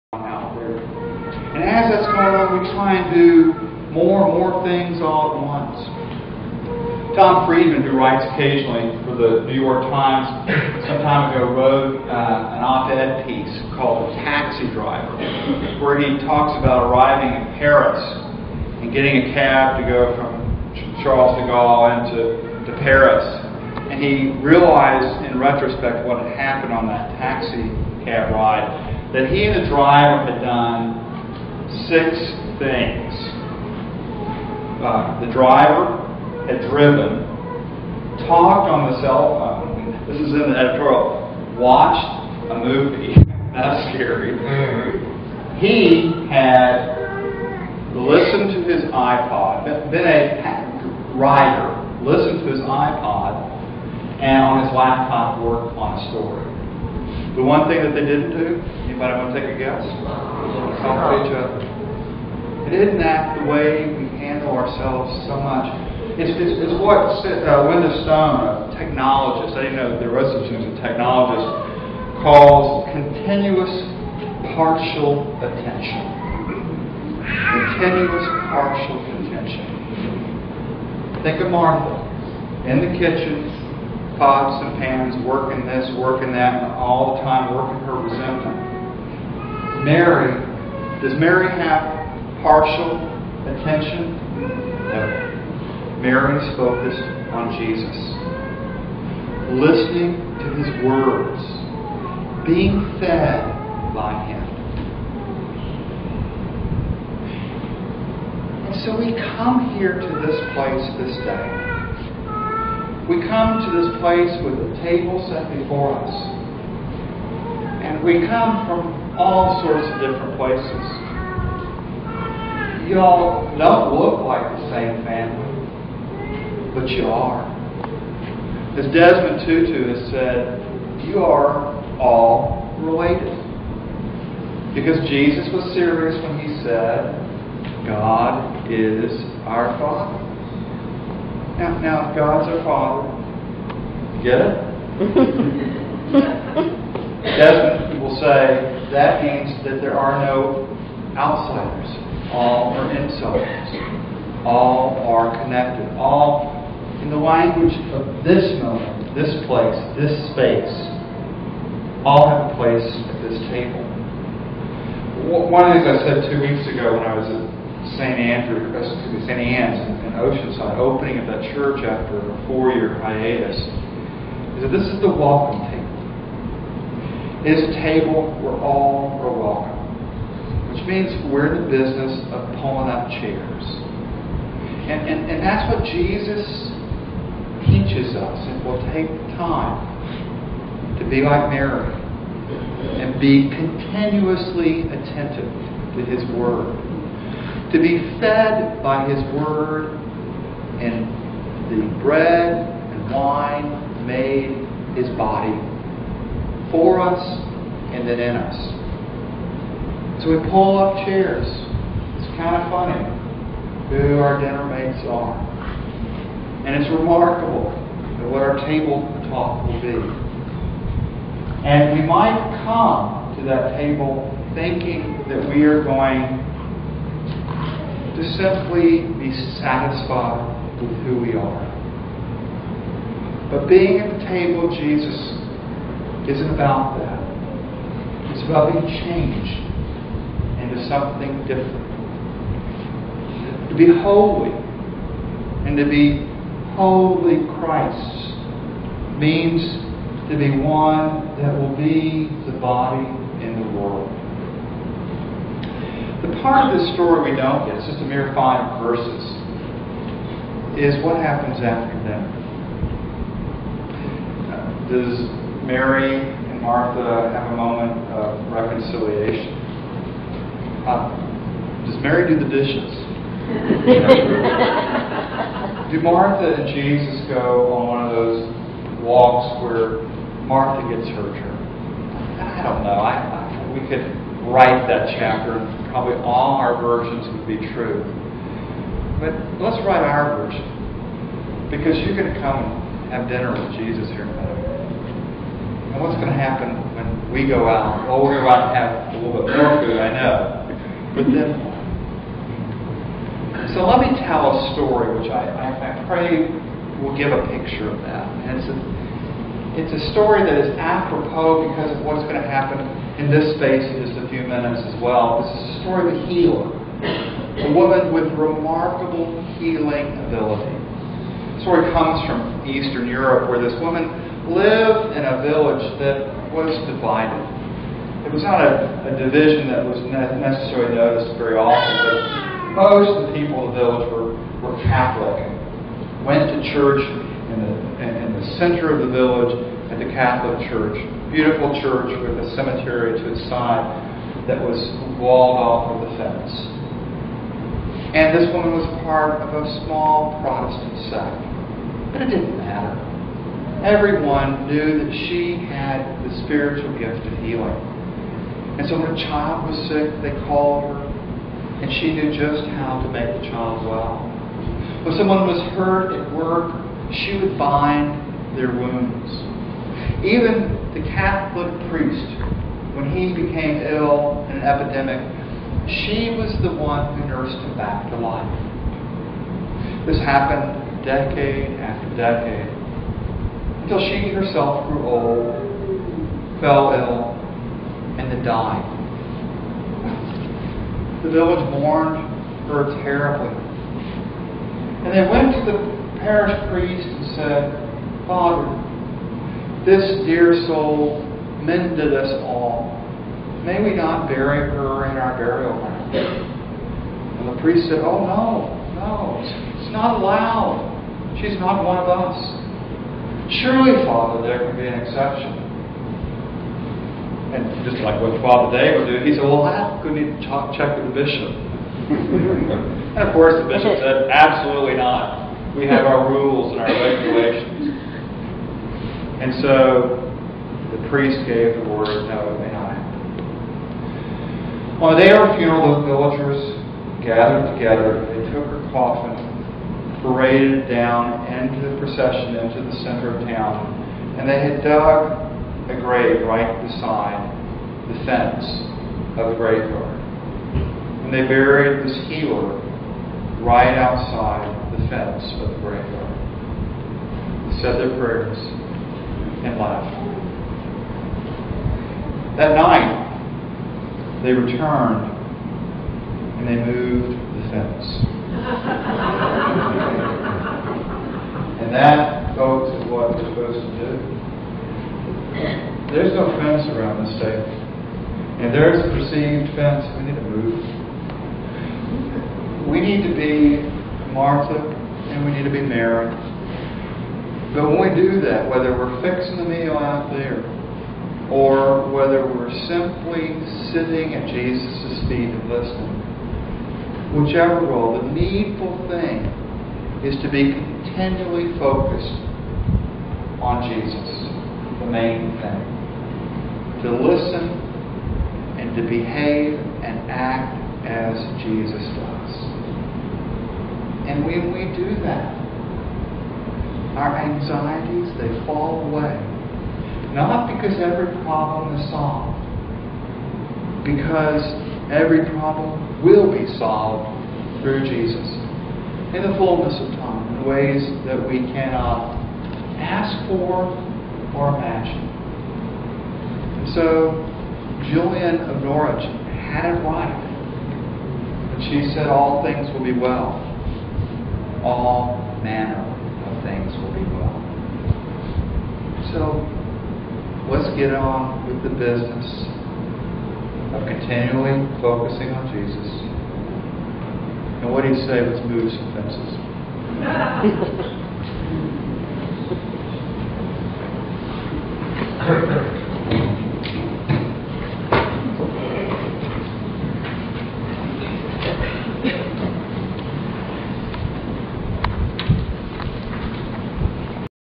Bishop's sermon 7.18.10
In these anxious times, remember Jesus' words - focus on the important things, those things that do not pass away.